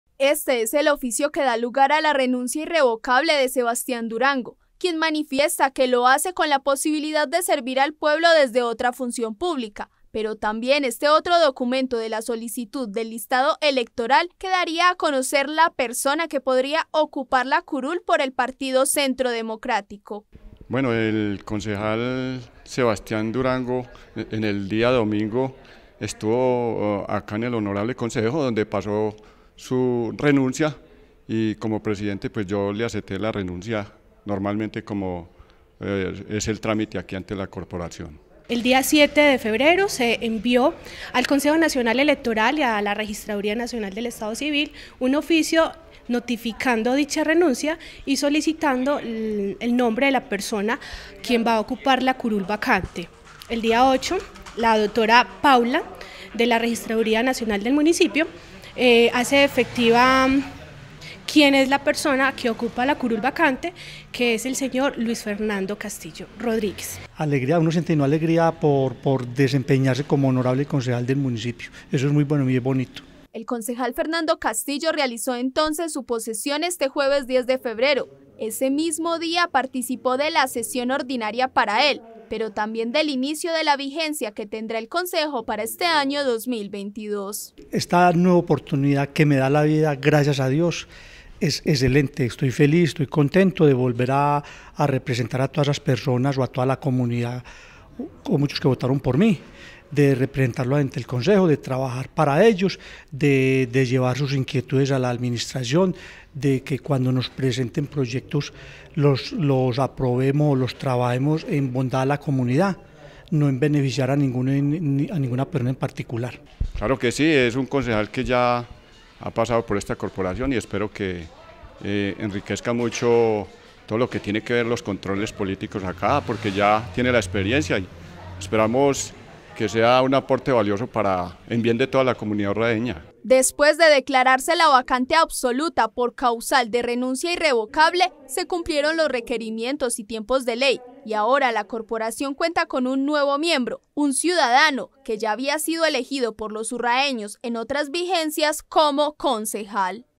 Renuncia Sebastián al Concejo, asume Castillo
El día jueves en el recinto del Concejo Municipal se llevó a cabo la posesión oficial de Luis Fernando Castillo como nuevo miembro de la corporación ante la renuncia del también miembro del Centro Democrático Sebastián Durango.